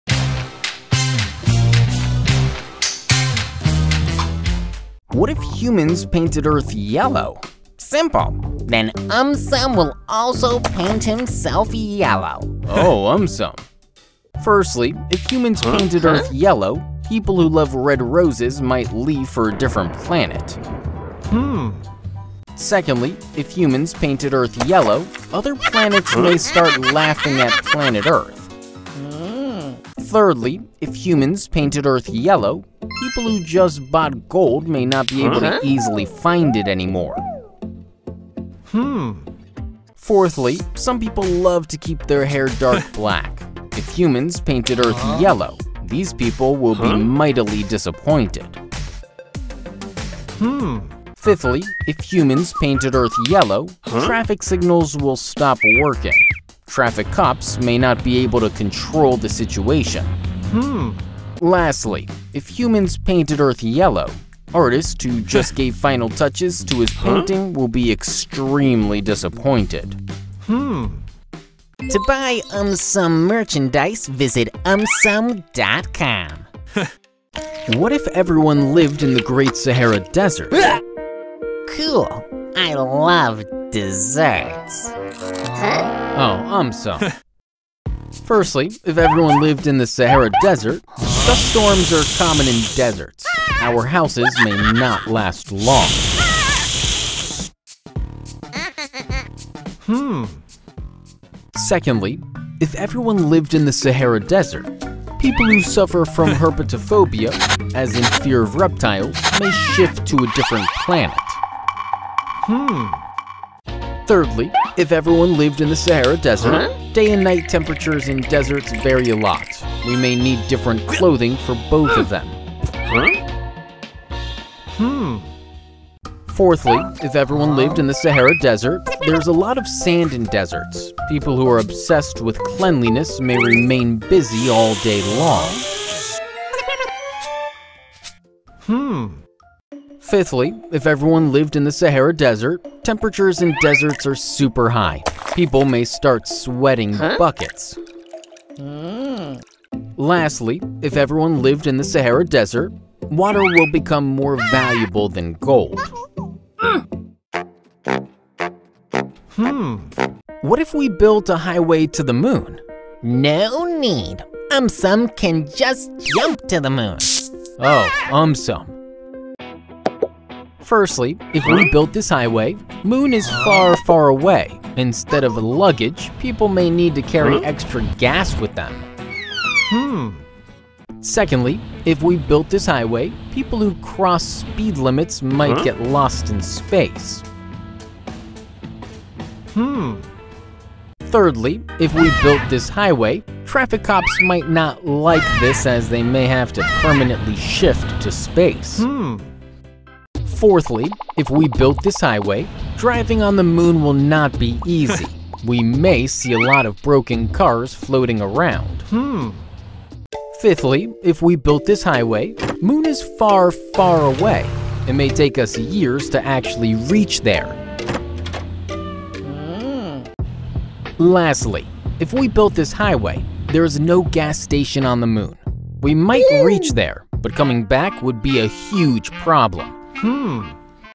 What if Humans painted Earth Yellow? + more videos
What if Humans painted Earth Yellow?
Firstly, if Humans painted Earth Yellow, people who love red roses might leave for a different planet.
Secondly, if Humans painted Earth Yellow, other planets may start laughing at planet Earth.
Thirdly, if Humans painted Earth Yellow, people who just bought gold may not be able to easily find it anymore.
Fourthly, some people love to keep their hair dark black, if Humans painted Earth Yellow, these people will be mightily disappointed.
Fifthly, if Humans painted Earth Yellow, traffic signals will stop working, traffic cops may not be able to control the situation.
Lastly, if Humans painted Earth Yellow, artist who just gave final touches to his painting will be extremely disappointed.

Timecodes
0:00 - What if Humans painted Earth Yellow?
1:17 - What if Everyone lived in the Great Sahara Desert?
2:40 - What if we built a Highway to the Moon?